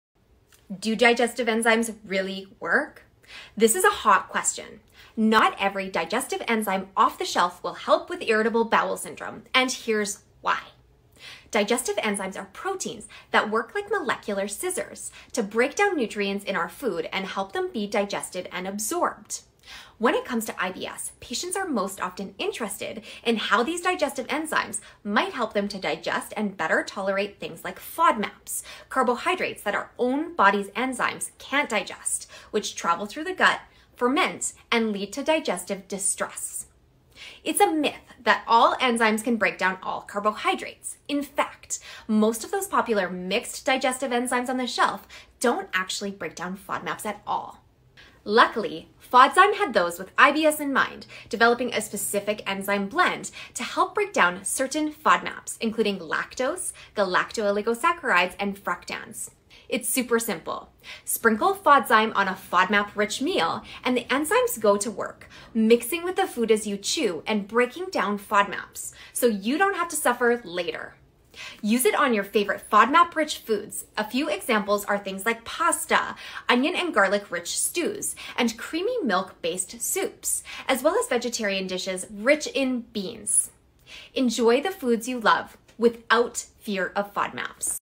Do digestive enzymes work? Andrea Hardy, RD reviews FODZYME® for IBS
Not all digestive enzymes can help with common gut symptom triggers like gas, diarrhea, abdominal pain, and constipation.

It's a myth that all digestive enzymes can break down all carbohydrates (including FODMAPs). Truth is, only specific enzymes can integrate with specific FODMAP groups (fructan, galactooligosaccharides (GOS), lactose).

An enzyme is a highly specialized protein that acts on a specific molecule, called the enzyme's substrate. You can think of enzymes as specialized molecular scissors.

Fructans (found in garlic, onion, wheat, banana, and more) can be broken down by fructan hydrolase, a key ingredient in FODZYME.

FODMAPs are short-chain carbohydrates that move through our bodies undigested. They function as healthy prebiotic fibers and promote good gut bacteria.

However, these carbohydrates are also poorly digested and can trigger gut symptoms like abdominal pain, constipation, excessive gas, bloating, and diarrhea.

With FODZYME®, you get the best of both: providing your body with the nutrients it needs, yet reducing the negative effect of FODMAPs.